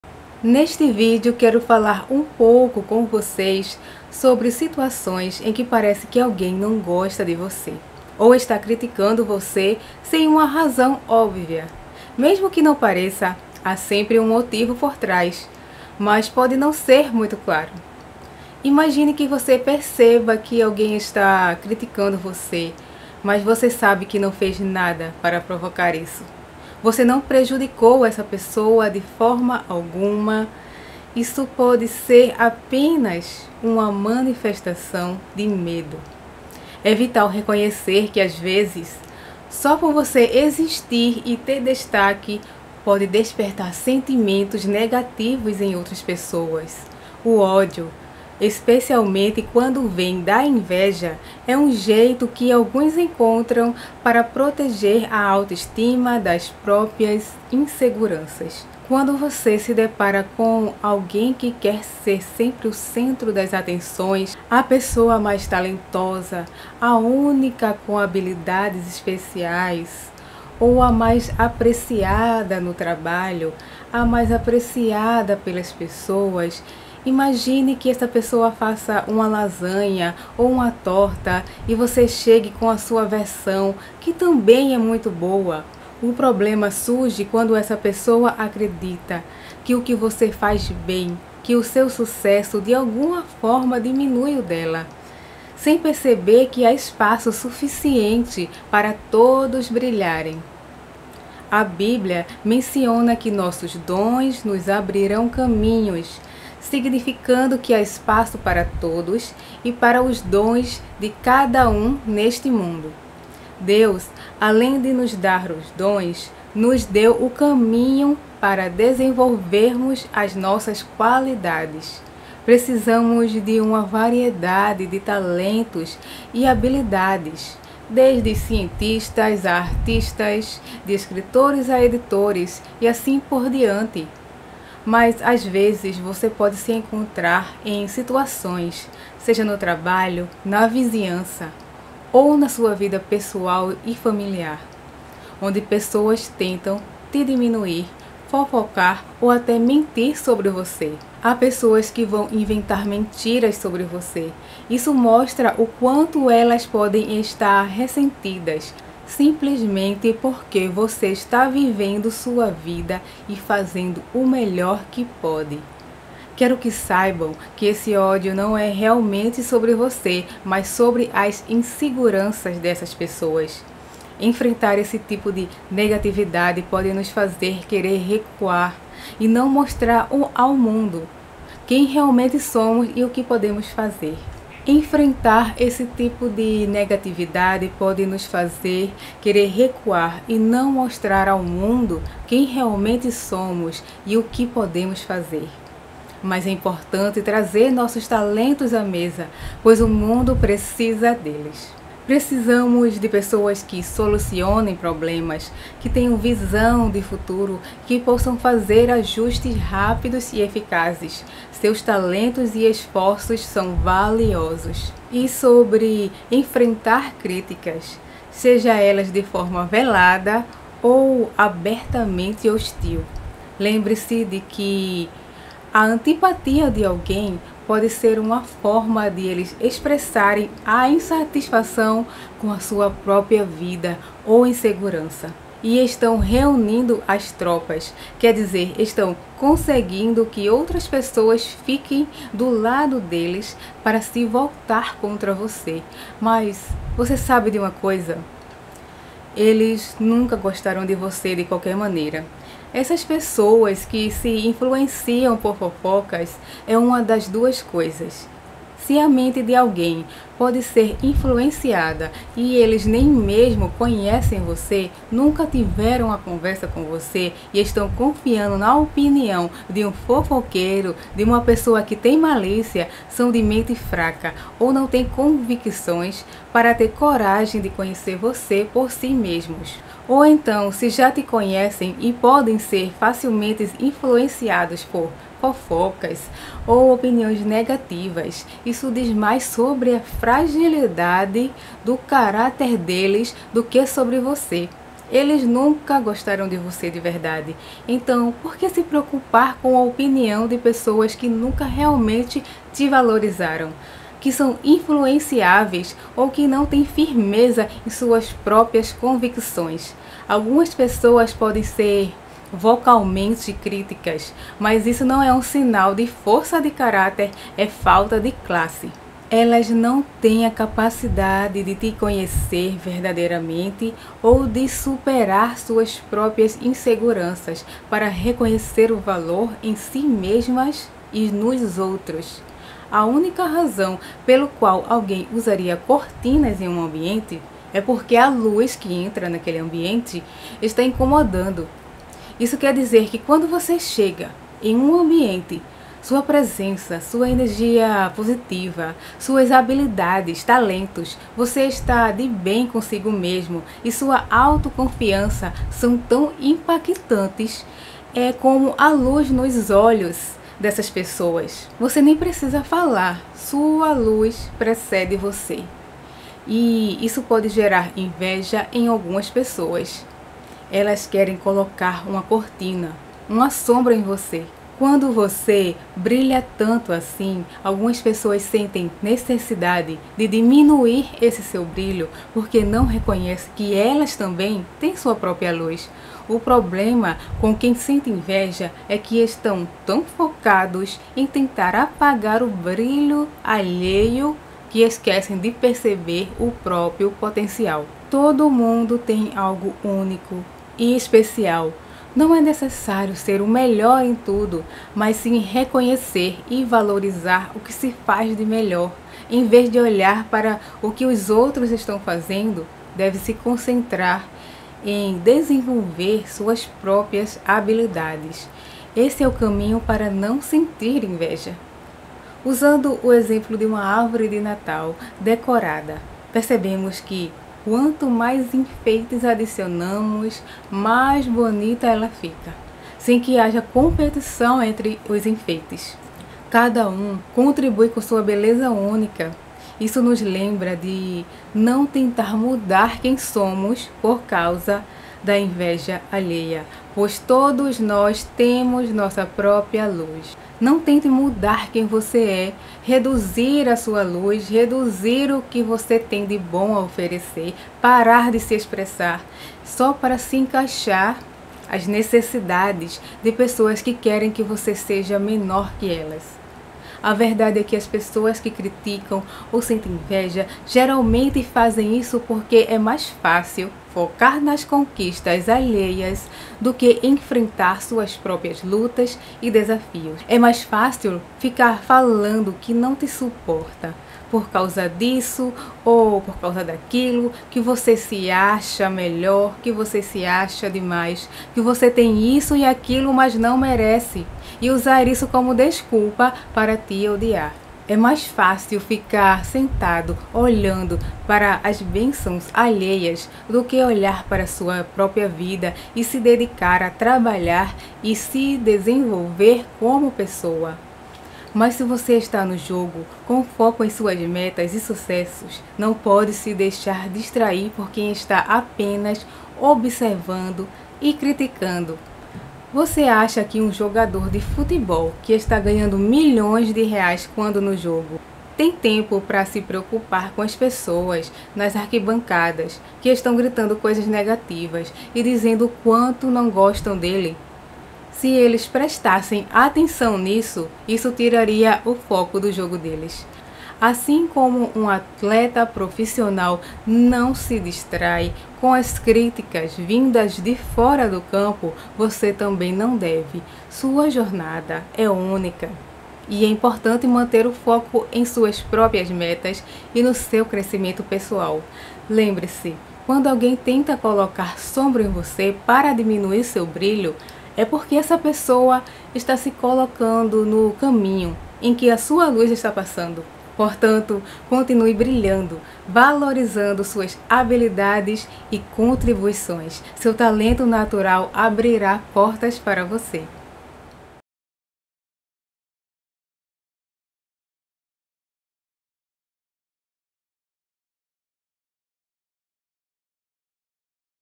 Se alguém tem raiva de você sem motivo é isso o que se esconde por trás...
Tento explicar o principal motivo quando você pensar: porque uma pessoa não gosta de mim sem motivo. Sem eu ter feito nada contra ela.